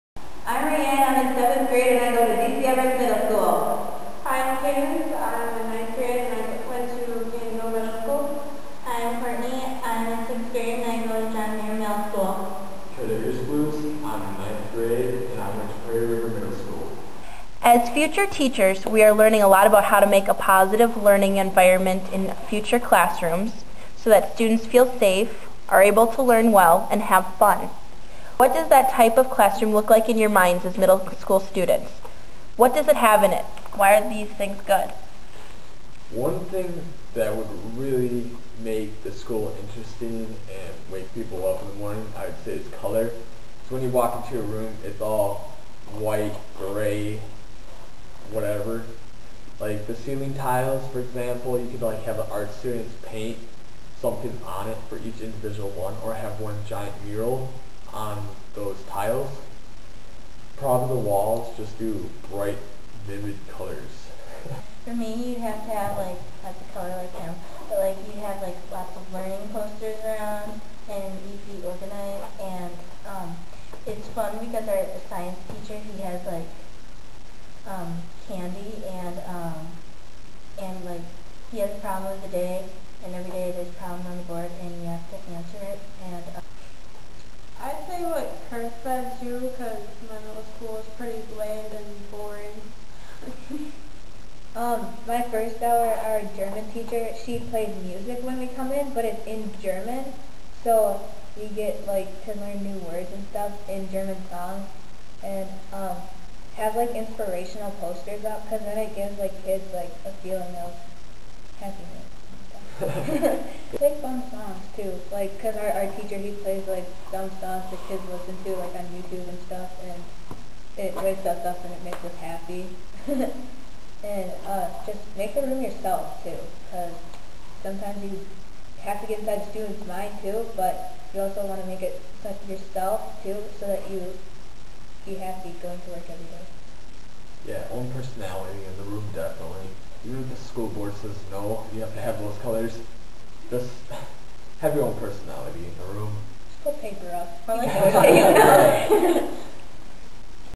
Positive Learning Environment
This is a video for our presentation section on Positive Learning Environments featuring some of my Figure Skating students.

Summer 2011.